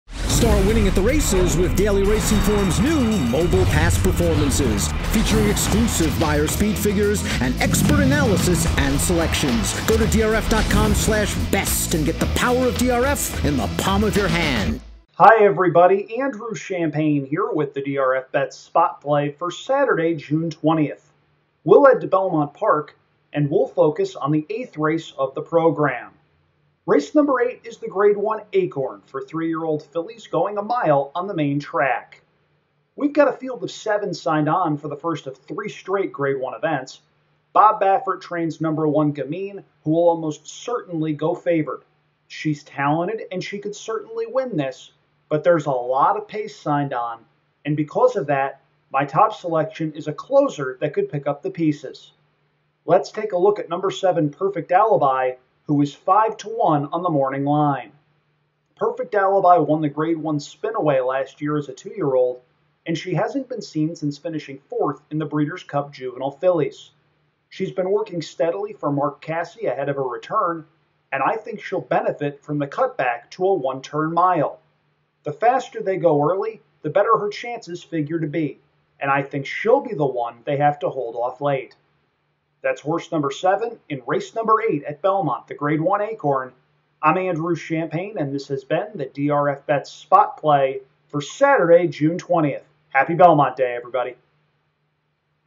DRFBets Spot Play | Acorn Stakes | June 20, 2020
Here's Andrew Champagne with a Spot Play for the Grade 1 Acorn Stakes from Belmont Park on Saturday on DRFTV